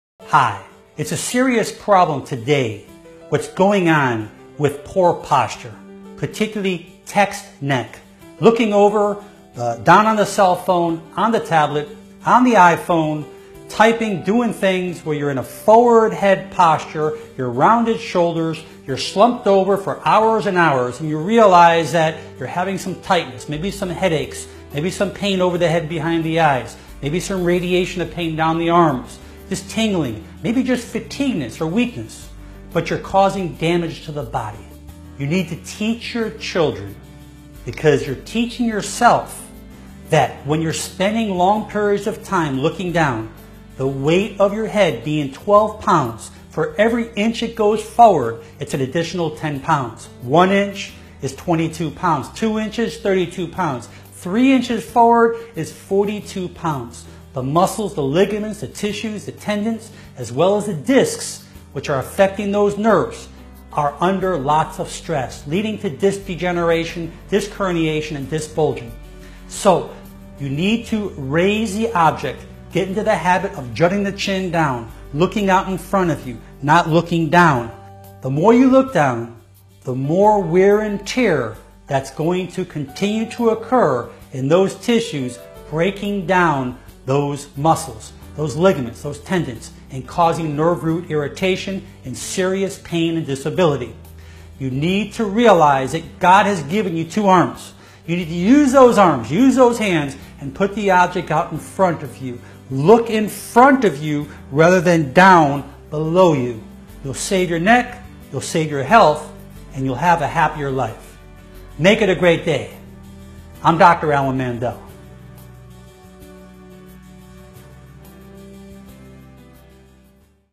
How Heavy is Your Head ?? - Text Neck Syndrome
'Text neck' - a condition often brought on by bending over phones and tablets for several hours at a time - is becoming an epidemic.

'The condition is called 'text neck' because it is often caused when people sit with their heads dropped forward looking at their devices for several hours at a time.

This video is by: Dr. Alan Mandell